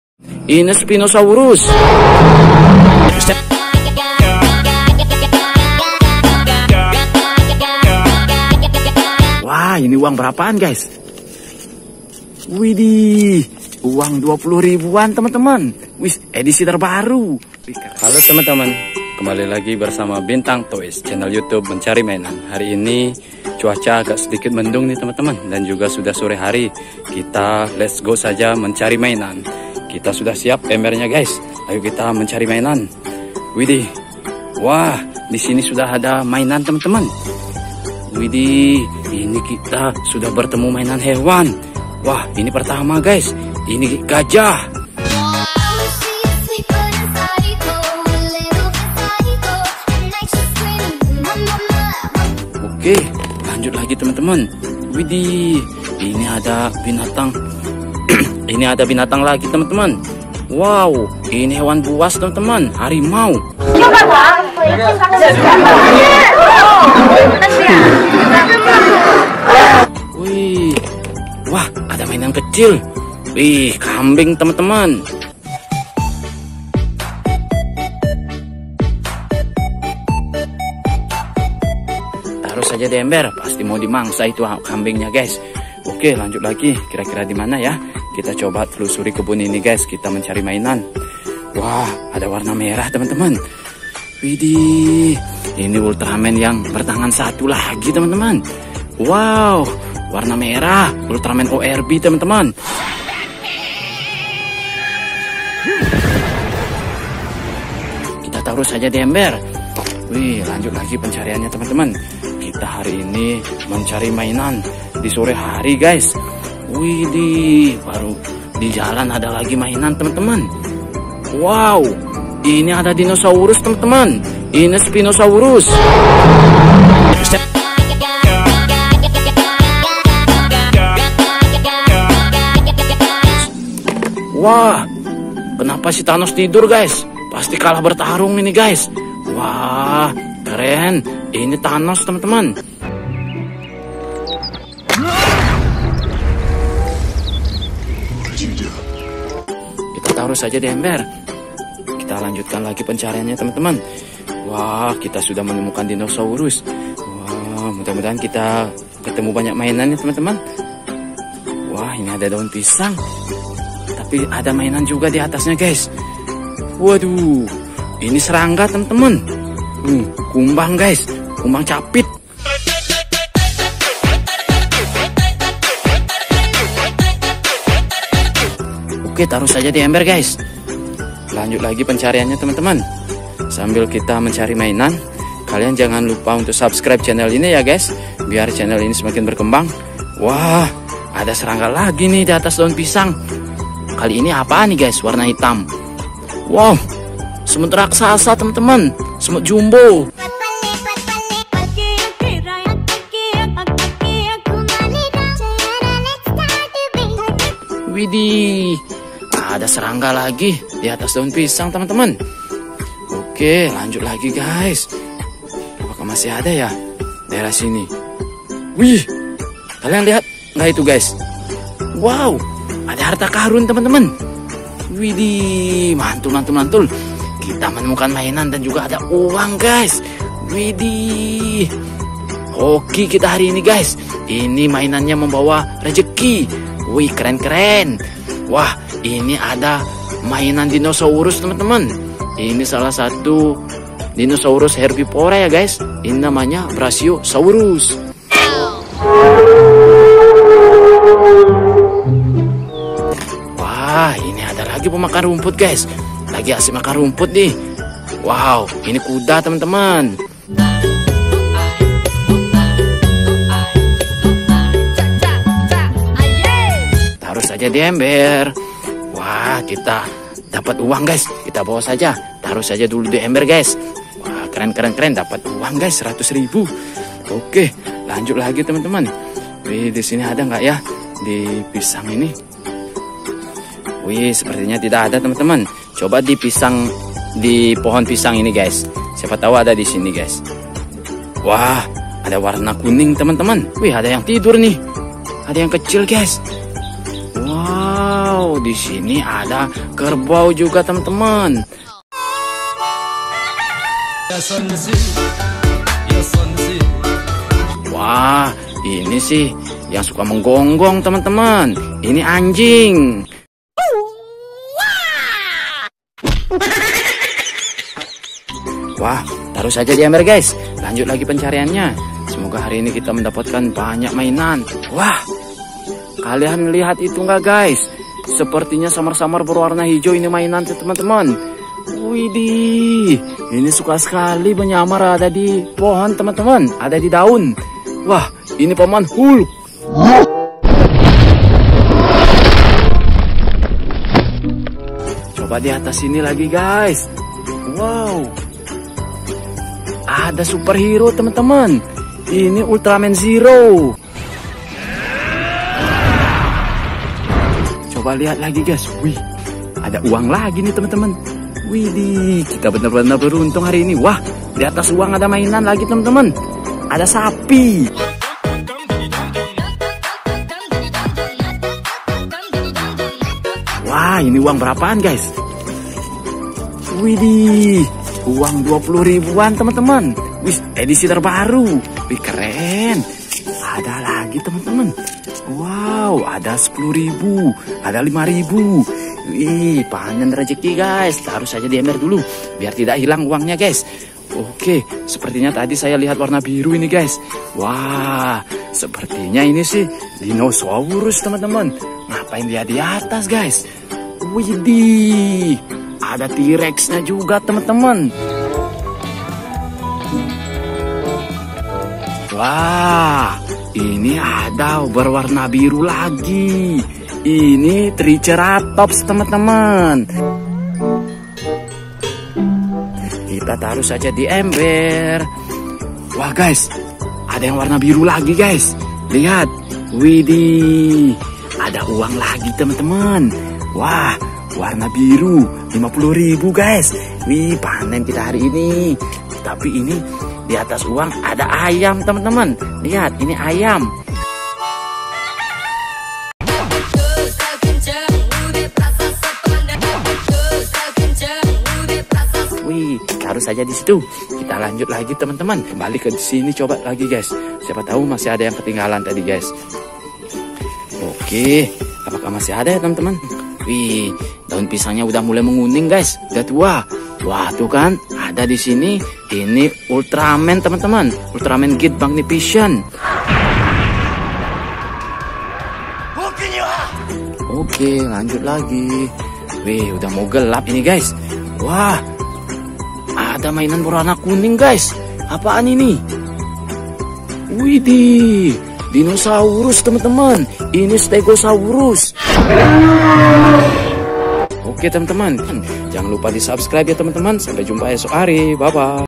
WOWW SERU!! Menemukan Harta Karun Uang Dan Mainan Ultraman Zero | Hunting Found Jurrasic World Ev: 2
terimakasih

thankyou